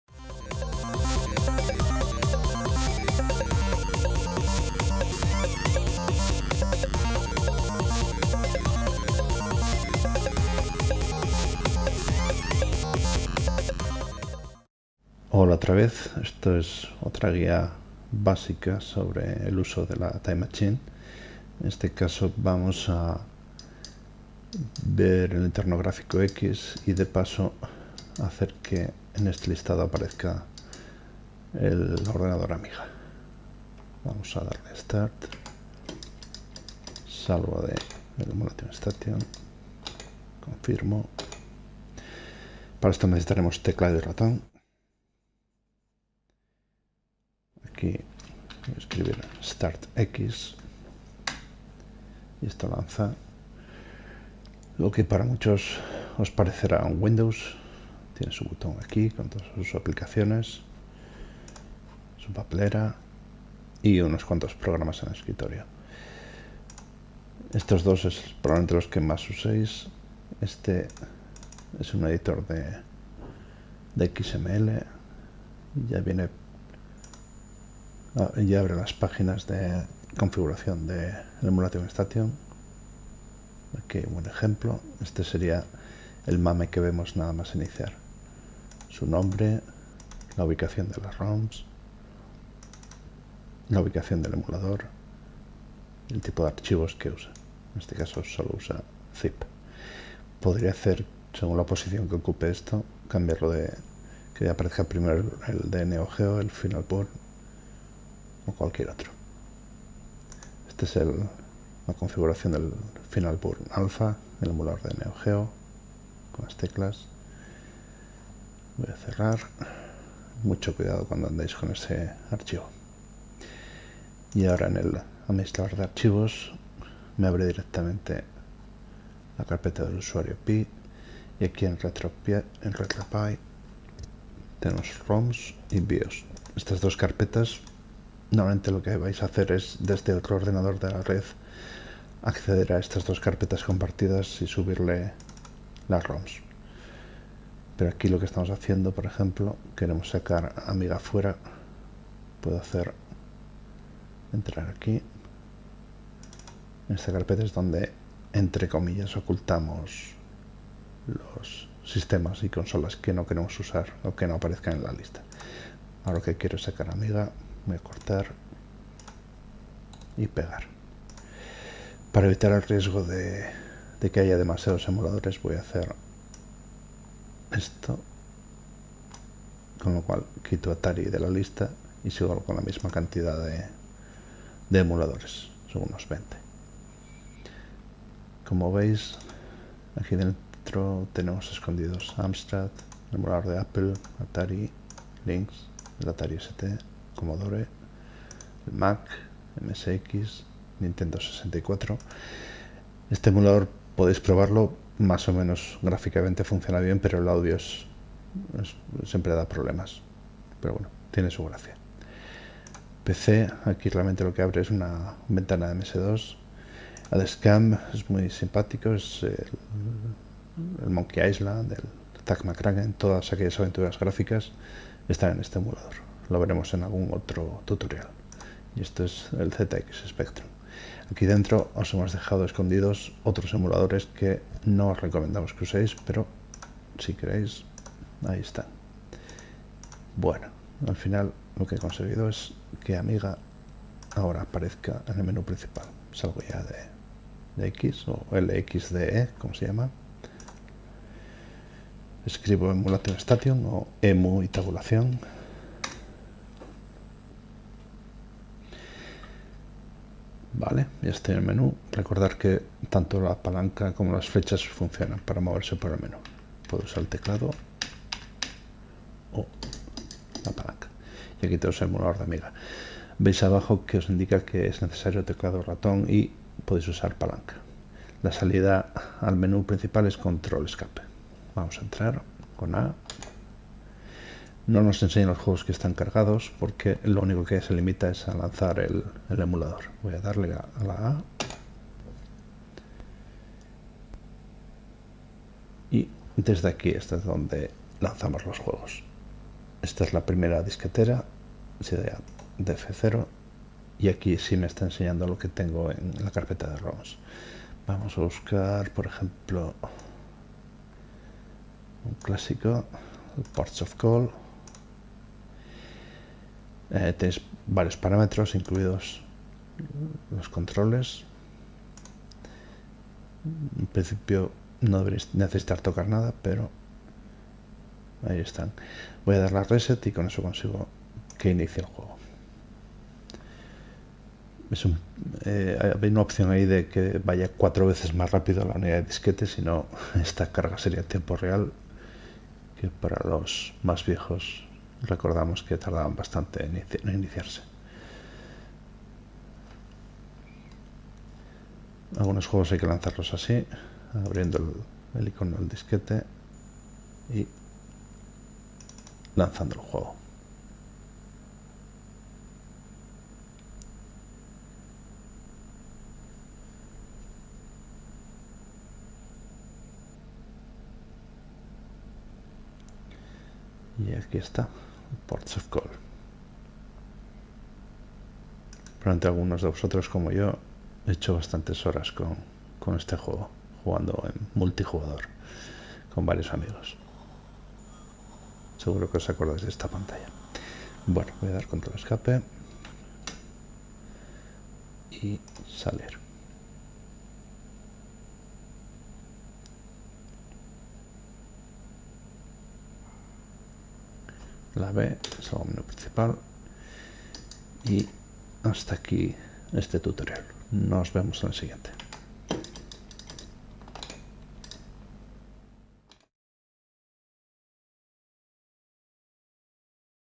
TOAD Time Machine Tutorial #2: Guía Avanzada de uso
(Parte de este contenido ha quedado obsoleto con las últimas versiones de nuestro software)

En este vídeo conoceremos funciones más avanzadas como incorporar nuevos emuladores usando como ejemplo el Commodore Amiga.